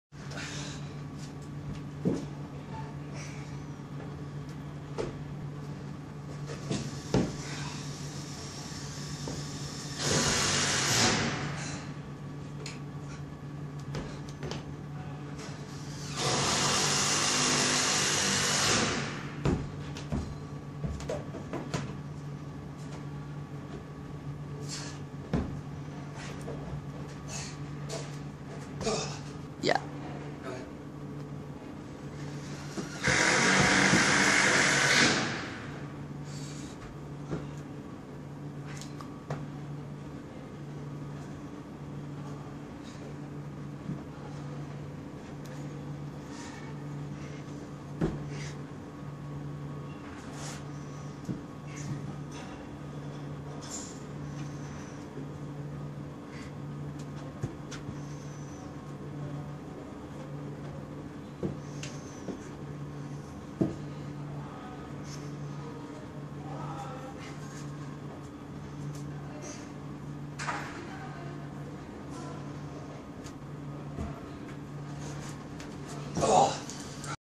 Boulder Wall Harlow
couple of V2 climbs at Harlow Climbing wall uk